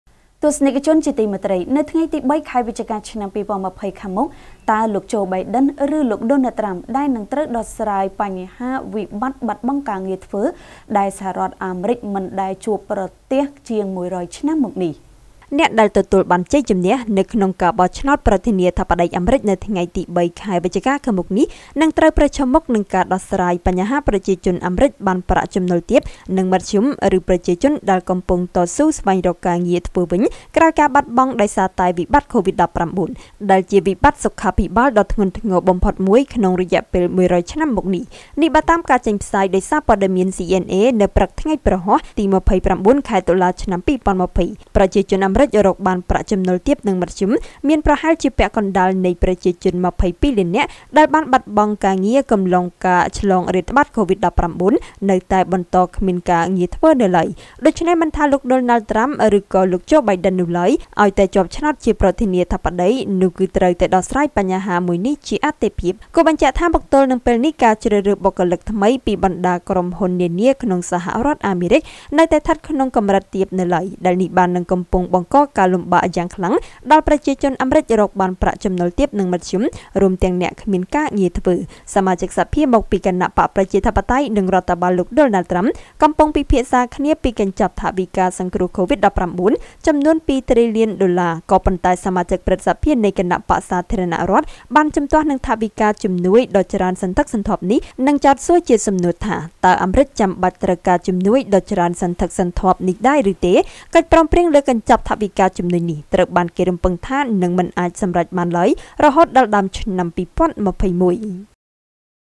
វាជាវិបត្តិបាត់បង់ការងារធ្វើ ដែលអាមេរិកមិនធ្លាប់ជួបប្រទះជាង១០០ឆ្នាំ, តើលោក បៃដិន ឬក៏លោក ត្រាំ ...
វាជាវិបត្តិបាត់បង់ការងារធ្វើ ដែលអាមេរិកមិនធ្លាប់ជួបប្រទះជាង១០០ឆ្នាំ, តើលោក បៃដិន ឬក៏លោក ត្រាំ ដែលនឹងត្រូវដោះស្រាយបញ្ហានេះ?
* រាយការណ៍ព័ត៌មានដោយកញ្ញា  យឹម ចាន់ស្រីវីន  និងកញ្ញា សំ ស្រីនីត , ទូរទស្សន៍ព័ត៌មាន Fresh News